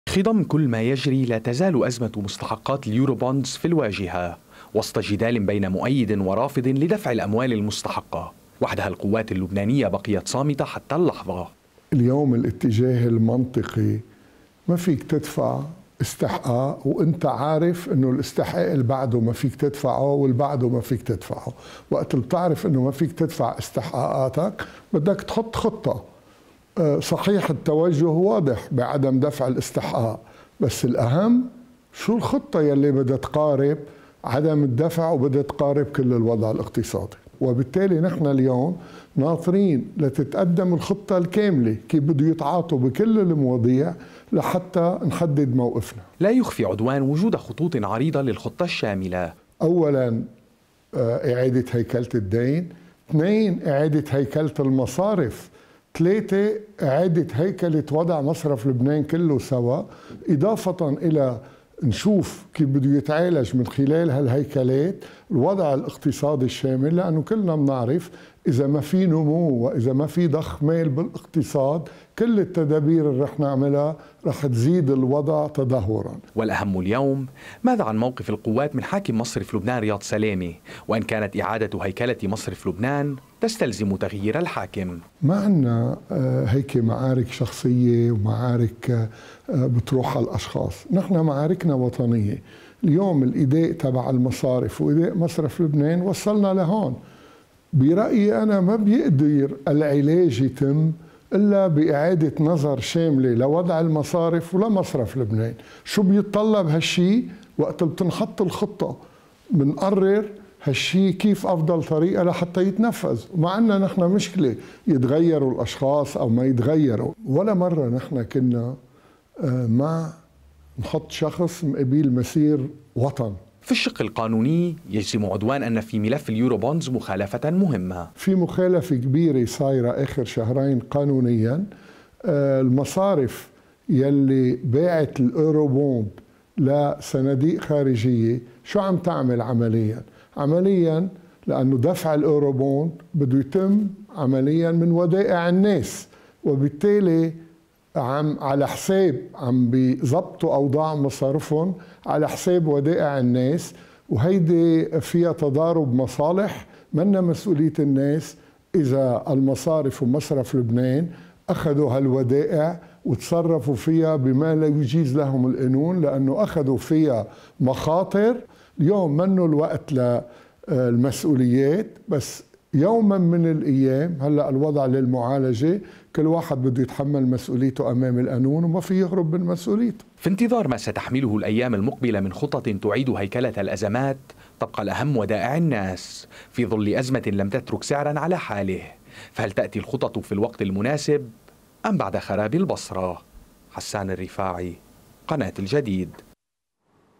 عدوان للجديد: آداء المصارف ومصرف لبنان أوصلنا إلى هنا - حسان الرفاعي
في خضم كل ما يجري، لا تزال أزمة مستحقات اليوروبوندز في الواجهة وسط جدال بين مؤيد ورافض لدفع الأموال المستحقة القوات اللبنانية بقيت صامتة حتى اللحظة لا يخفي عدوان وجود خطوط عريضة للخطة الشاملة والأهم اليوم، ماذا عن موقف القوات من حاكم مصرف لبنان رياض سلامة، وإن كانت إعادة هيكلة مصرف لبنان تستلزم تغيير الحاكم في الشق القانوني يجزم عدوان أن في ملف اليوروبوندز مخالفة مهمة في انتظار ما ستحمله الايام المقبلة من خطط تعيد هيكلة الأزمات تبقى الأهم ودائع الناس في ظل أزمة لم تترك سعرا على حاله فهل تأتي الخطط في الوقت المناسب أم بعد خراب البصرة؟